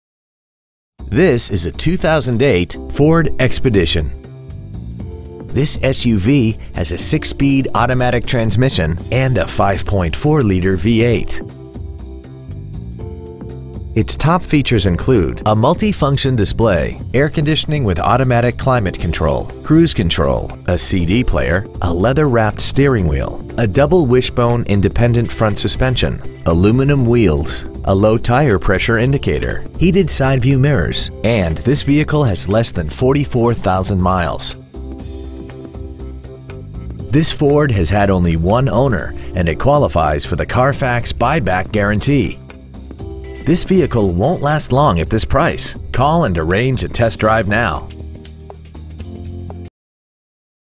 Preowned 2008 Ford Expedition Coconut Creek FL
Year : 2008

 Make : Ford

 Model : Expedition

 Engine : Gas V8 5.4L/330

 Trans . : Automatic

 Exterior : Black

 Miles : 43,372

 Interior : Charcoal Black

 Stock : 29608

 Offleaseonly.com

 3531 Lake Worth Rd

 Preowned 2008 Ford Expedition Lake Worth FL